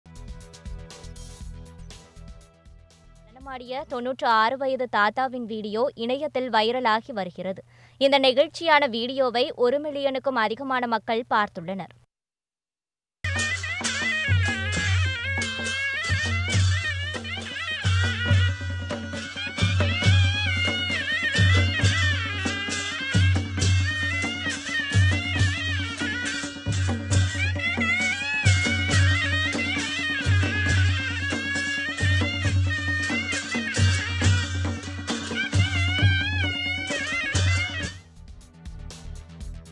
பேரனின் திருமணத்தில் 96 வயது தாத்தா துள்ளிக்குதித்து உற்சாக நடனம்! | Sathiyamtvnews
பேரனின் திருமணத்தில் 96 வயது தாத்தா துள்ளிக்குதித்து உற்சாக நடனம்!